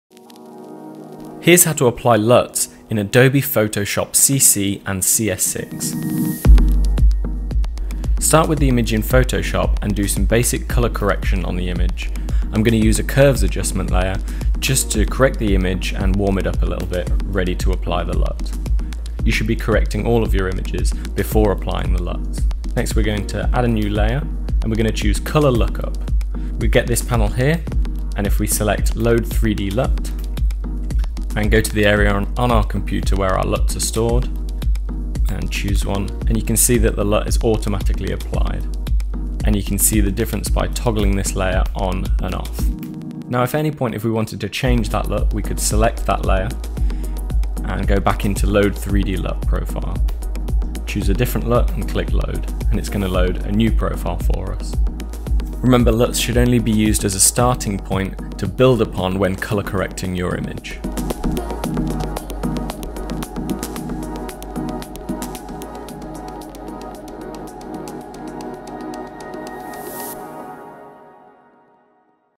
Using LUTs in Adobe Photoshop CS6 / CC
A quick look at how to use LUTs (Lookup Tables) in Adobe Photoshop CC. This also works in Adobe Photoshop CS6.

Download LUTs and other digital tools from The Preset Factory's Product Suite